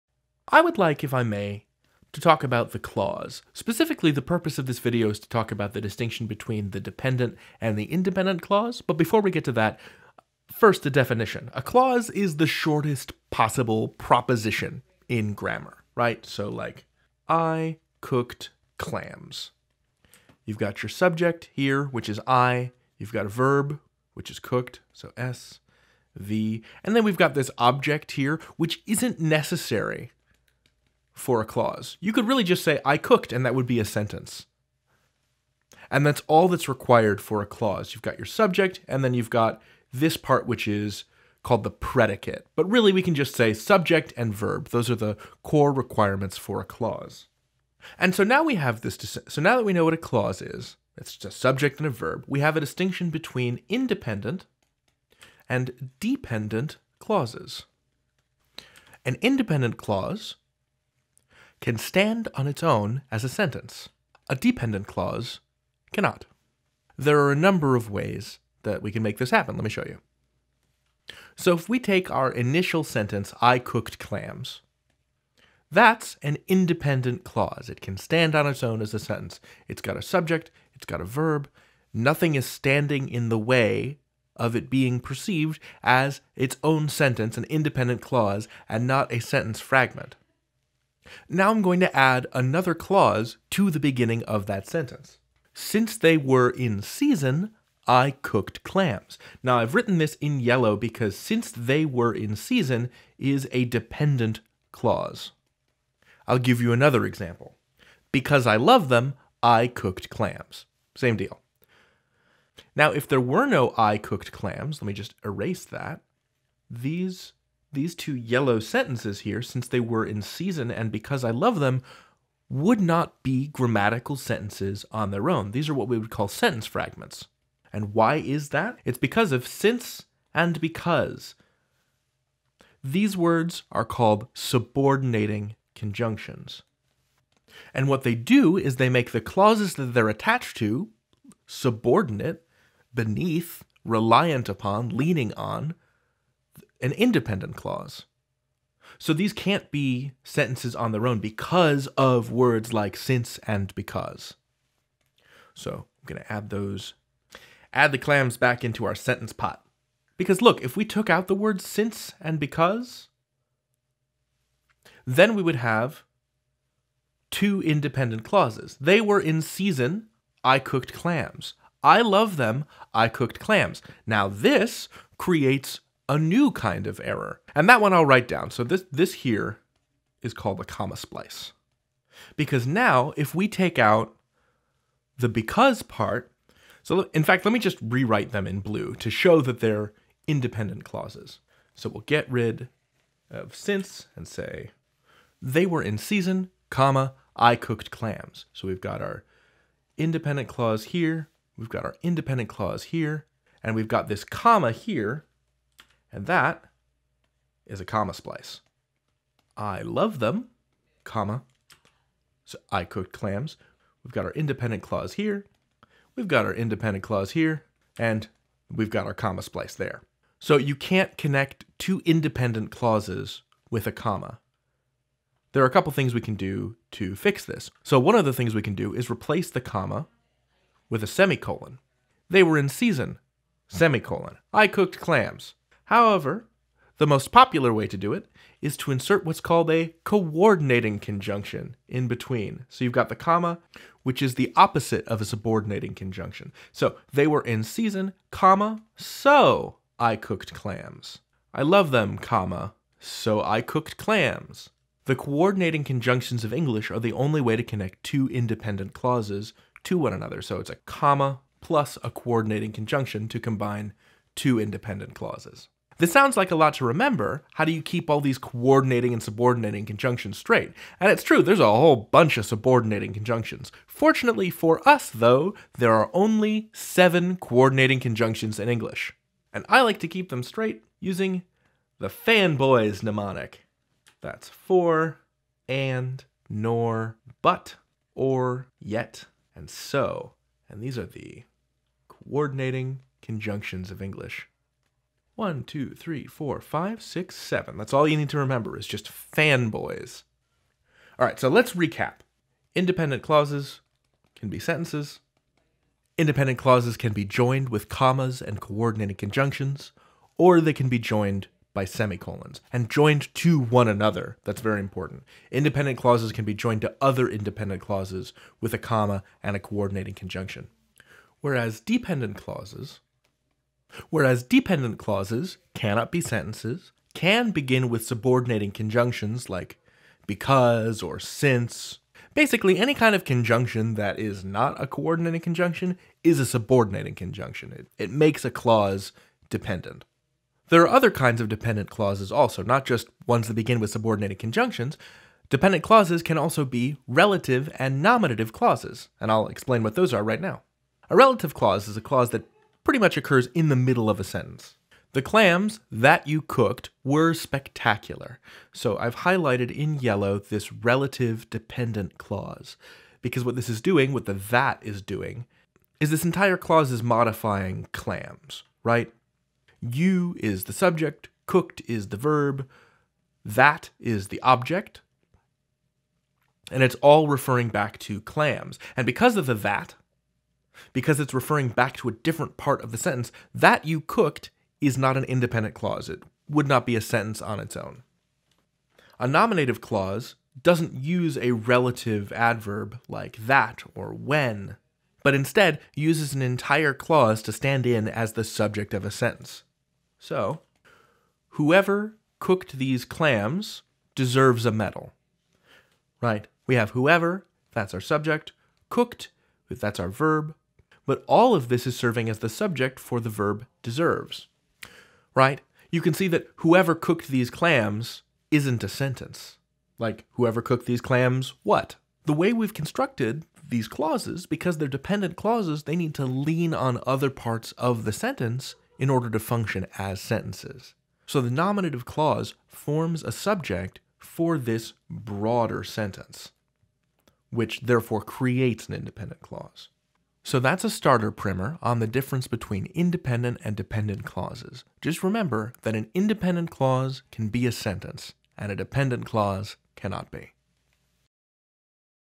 Independent and Dependent Clauses | Praxis Core Writing | Khan Academy
Watch David explain the difference between independent and dependent clauses, as well as the various types of dependent clauses.